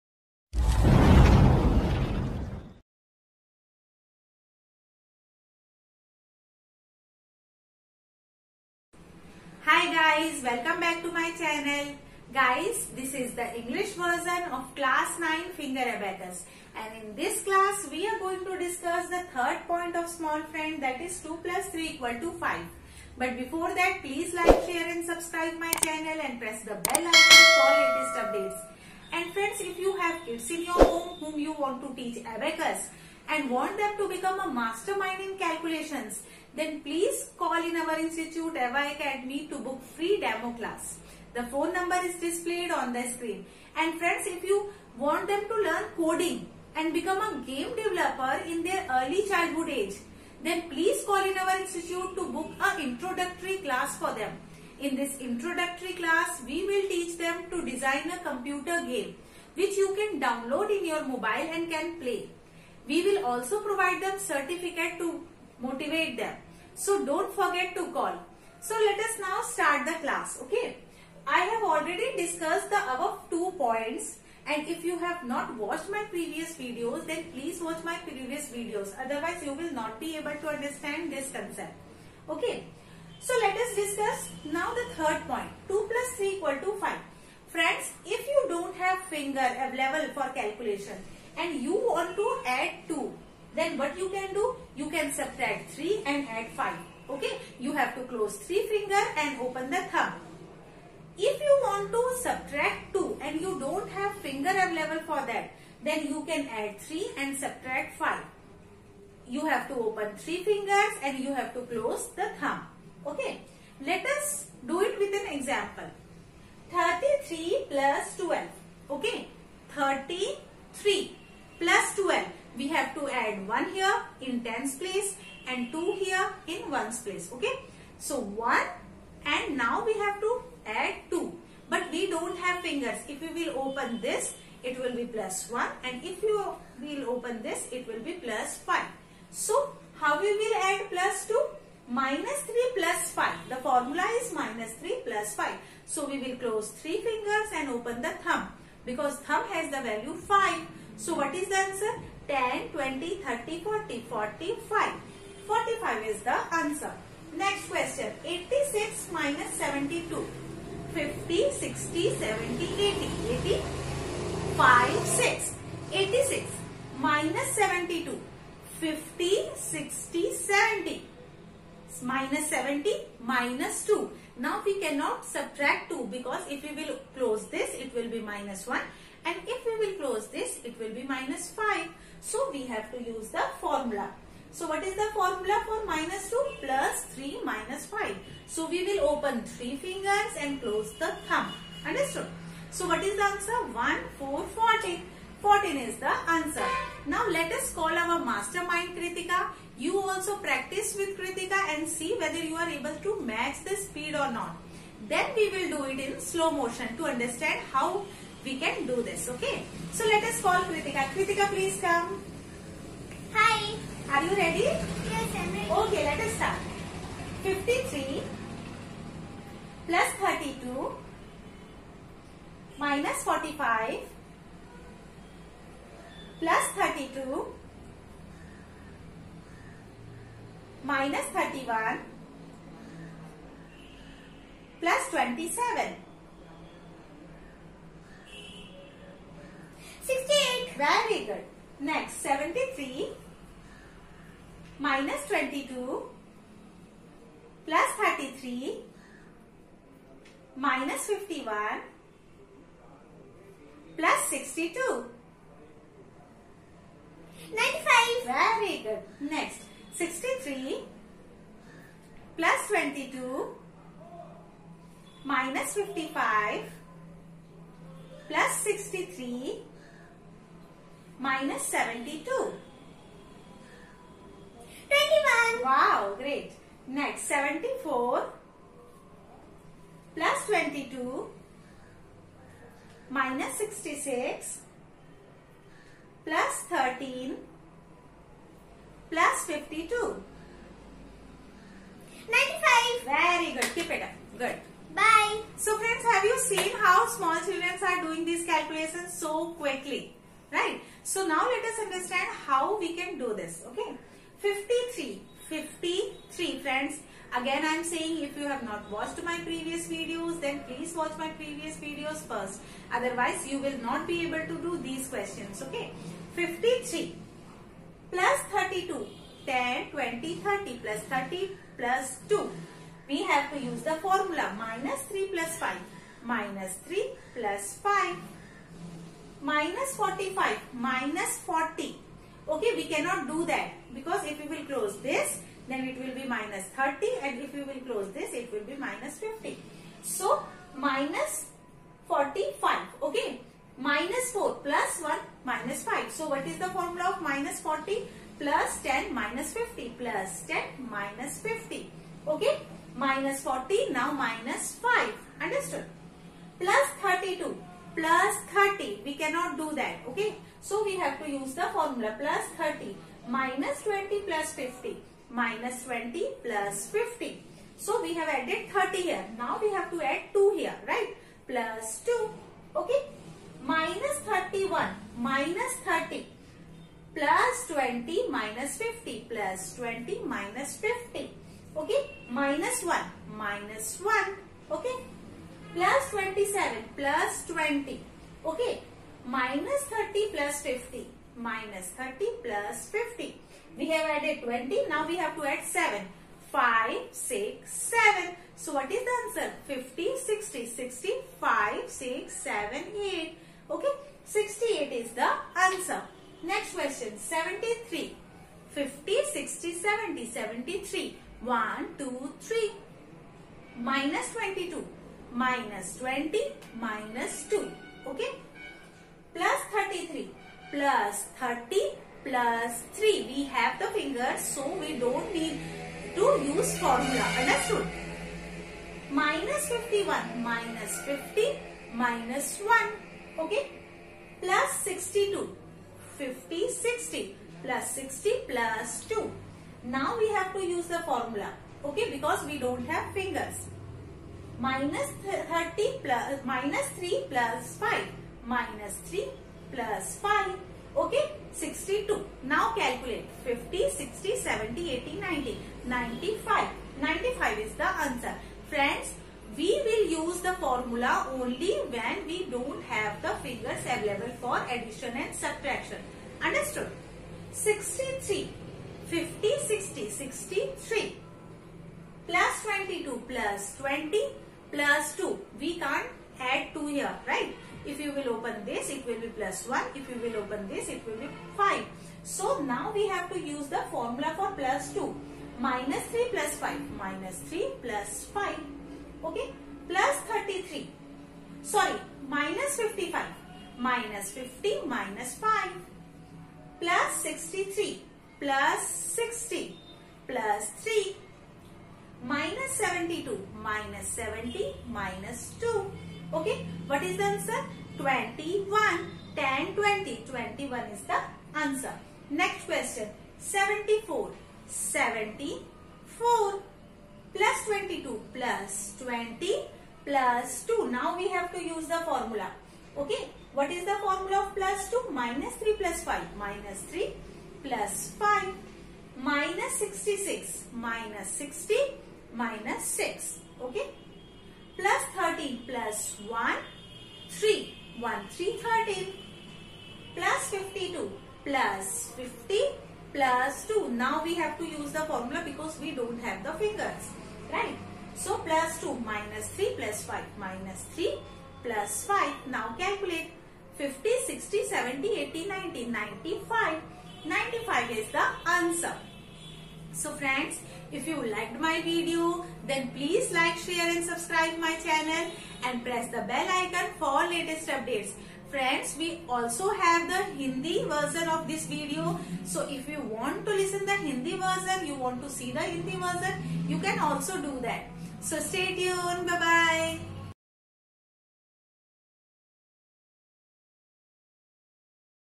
Finger Abacus | Abacus Class 9 | English | Unbelievably Fast Calculations By Small Kids
we love to teach and we are teaching finger abacus in easy way